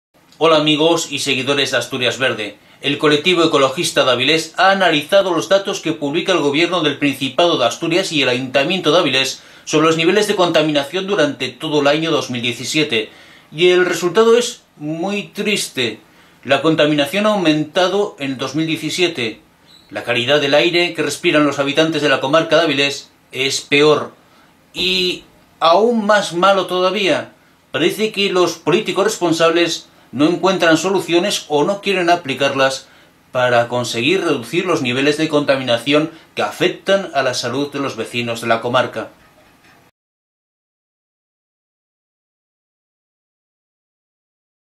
El Colectivo Ecologista de Avilés alerta que en 2017 volvió a subir la contaminación en la comarca
AsturiasVerde.com TV: Esto no va bien: Análisis anual en 2017 volvió a subir la contaminación en la comarca de Avilés